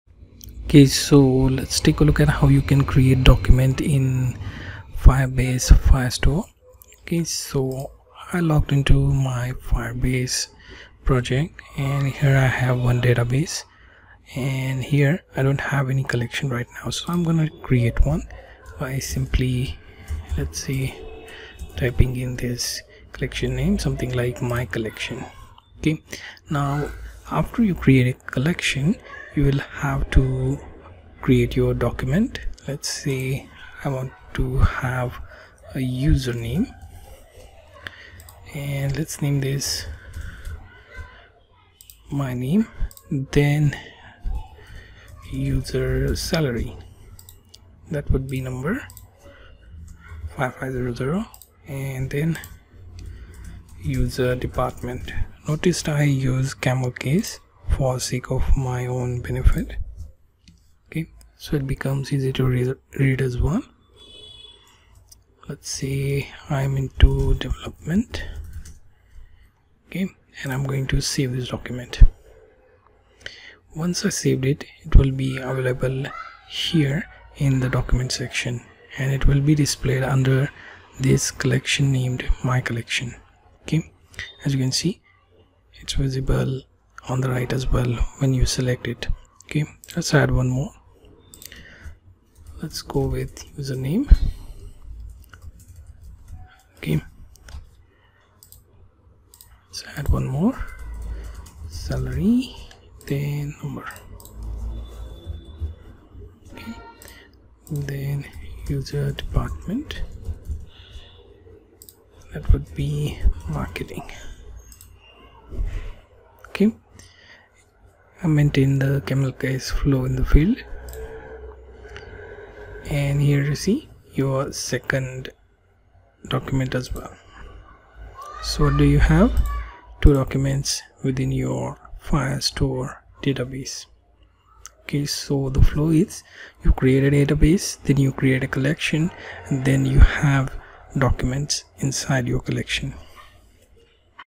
Firebase - How to Create Document in Cloud Firestore Database
Learn How to Create Document  Cloud Firestore Database in Firebase. Learn how to create document in Firebase Cloud Firestore.